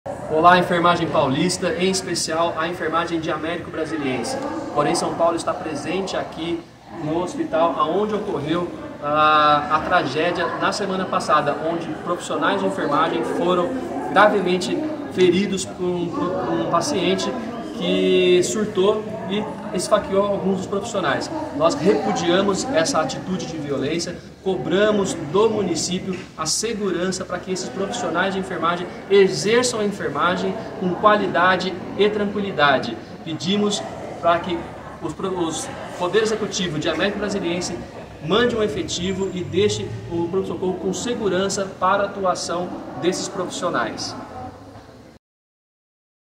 Conselheiros se reúnem com profissionais agredidos em Américo Brasiliense
O Coren-SP esteve na cidade de Américo Brasiliense, onde no dia 6/4 um homem esfaqueou sete pessoas no Hospital Municipal José Nigro Neto, inclusive profissionais de enfermagem. Felizmente nenhum dos atingidos se feriu com gravidade.

Os conselheiros Luciano Santos, Anderson Rodrigues e Edson José da Luz fizeram uma visita de apoio e acolhimento aos profissionais de enfermagem do hospital e reuniram-se também com representantes da Secretaria Municipal de Saúde e do Sindicato dos Servidores Municipais de Araraquara e Região. Os conselheiros representaram a Comissão de Relações Institucionais (CRI) do Coren-SP.